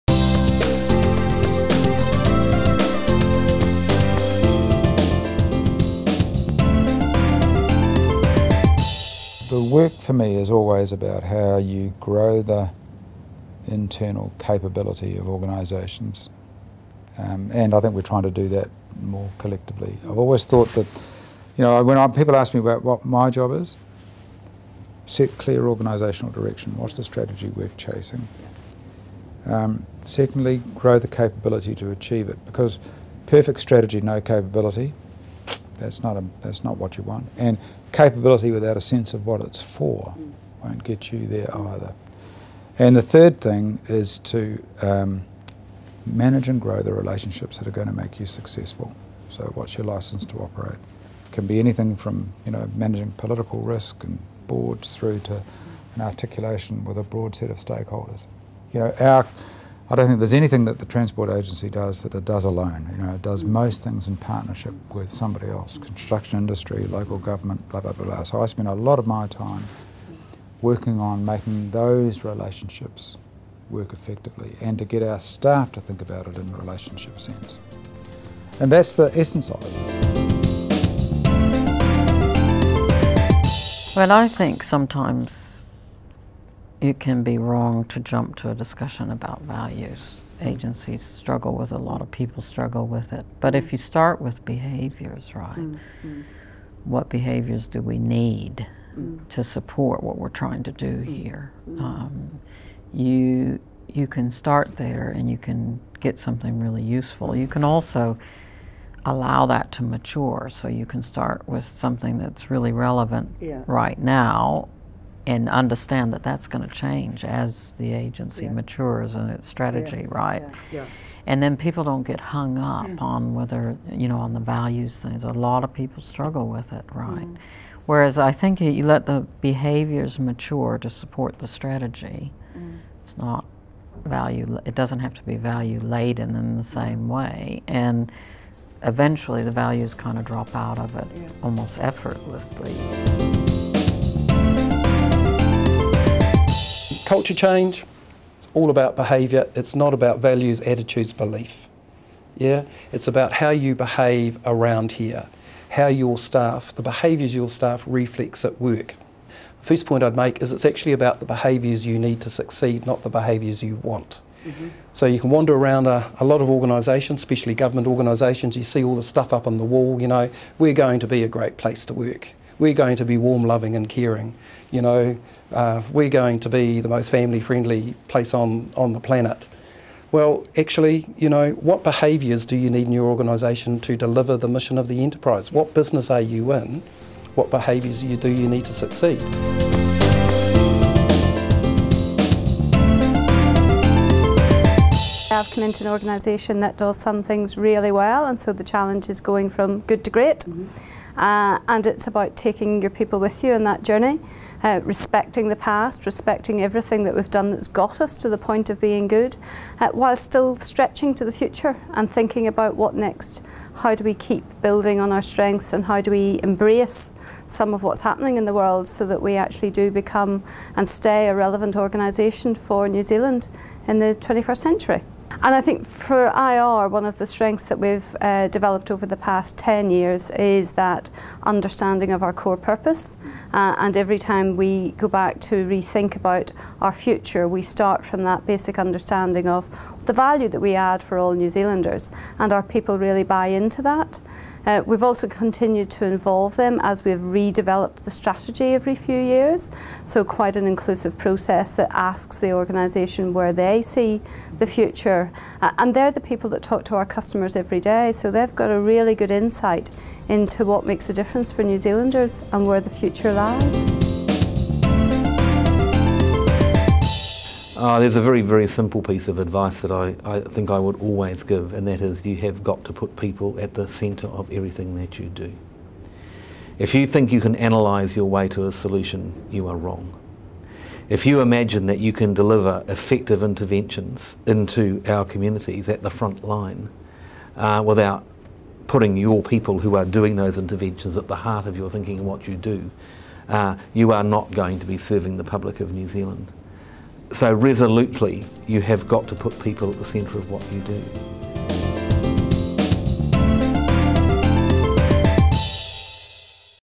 Leading culture change
Leading culture change. This video is part of the Performance Improvement Framework's Getting to Great: your map to navigating the straits of internal leadership.